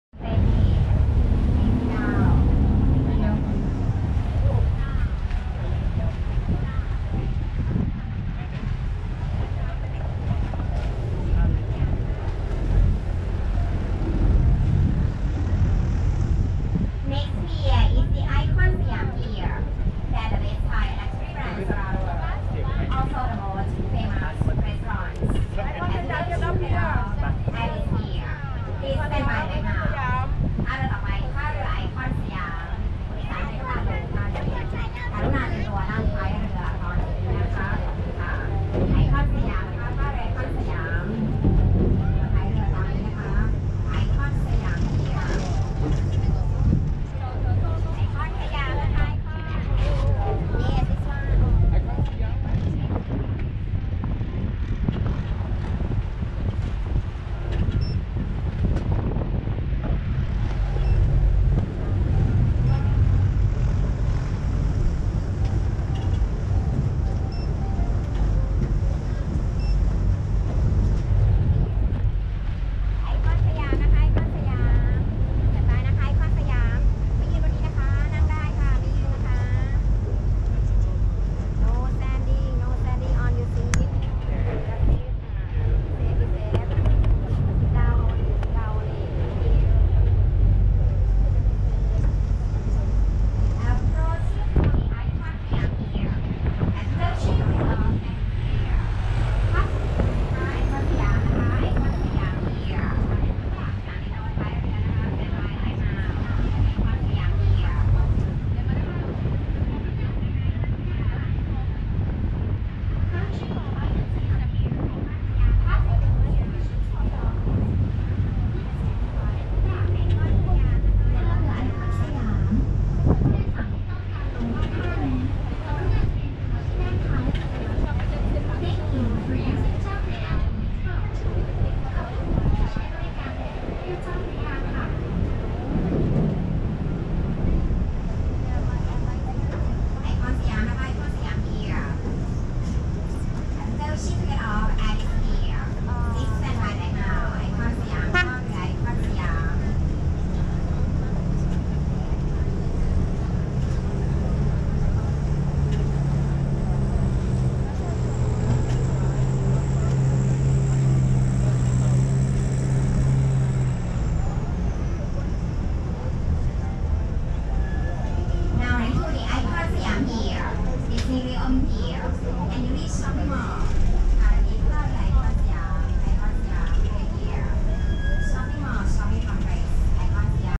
Chaophraya tourist boat
นั่งเรือแม่น้ำเจ้าพระยา บรรยากาศดี แต่บนดาดฟ้าเต็ม เลยไม่ได้นั่ง วันหลังต้องนั่งให้ได้